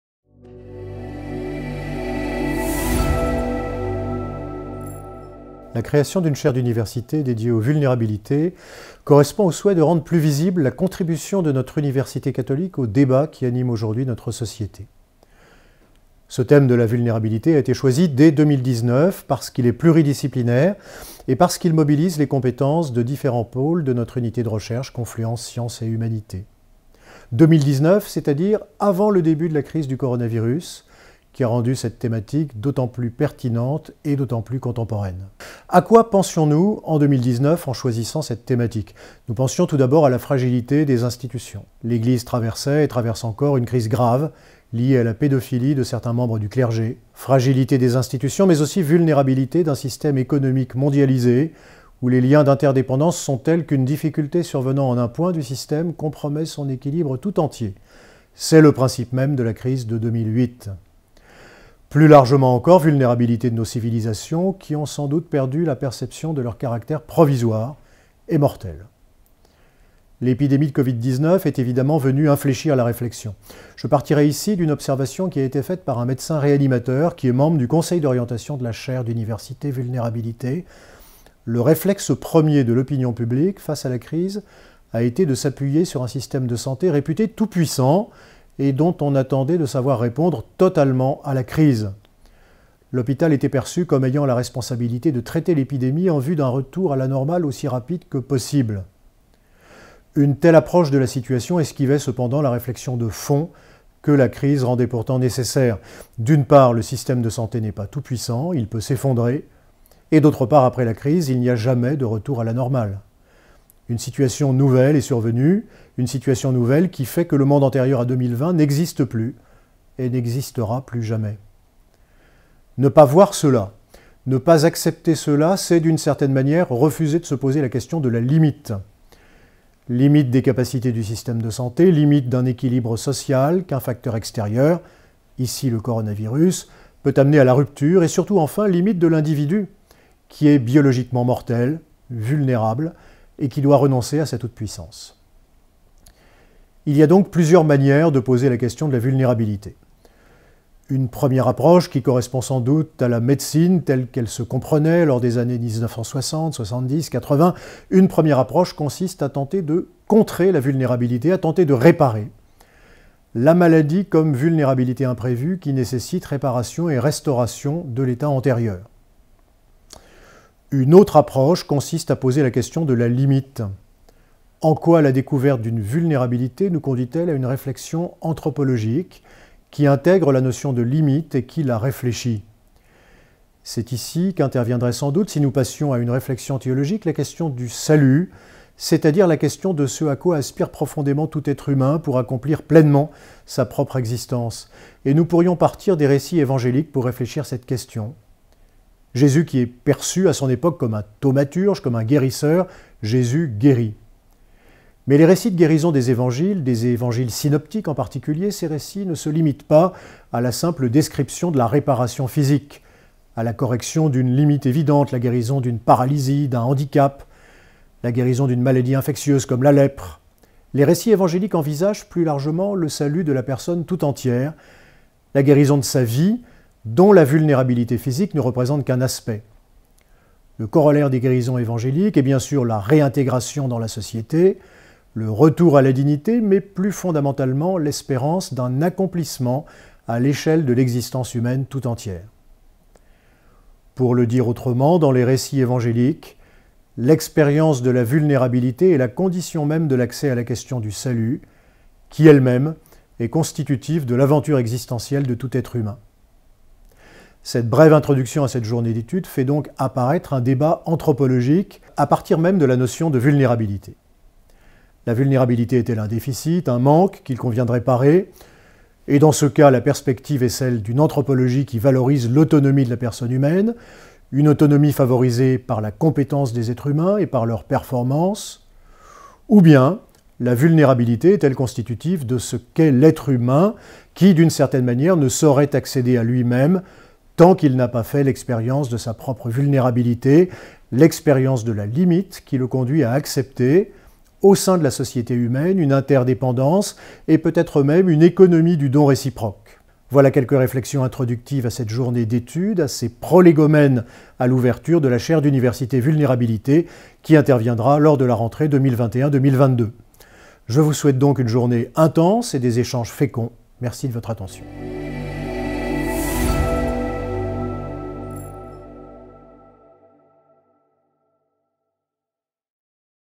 Propos introductifs à la Journée d'étude Covid et Vulnérabilité - Olivier Artus, Recteur de l'UCLy
Propos introductifs du Pr. Olivier Artus, Recteur de l'UCLy et Professeur en théologie, lors de la Journée d'étude interne de l'Unité de Recherche CONFLUENCE : Sciences & Humanités - UCLy